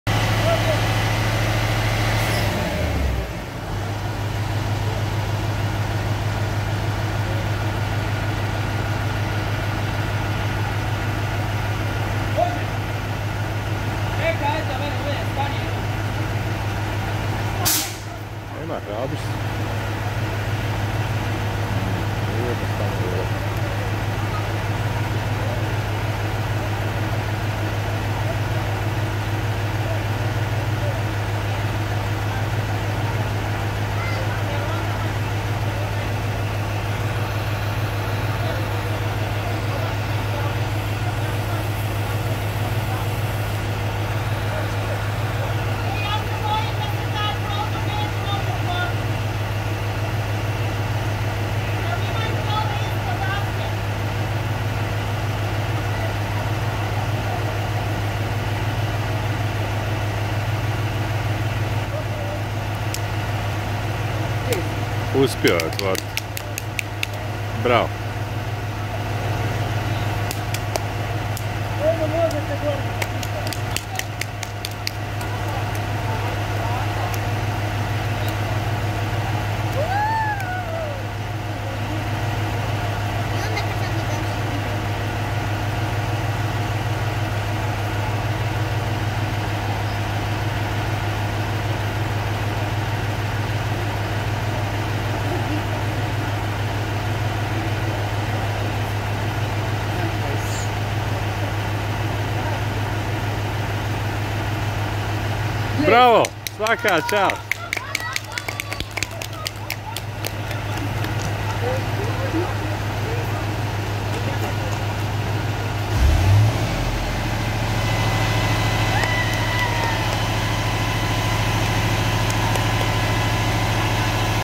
Vatrogasci u akciji. Bez razbijanja i obijanja, uspješno ušli u zaključani stan.
Uspješan ulazak u stan bez razbijanja vrata.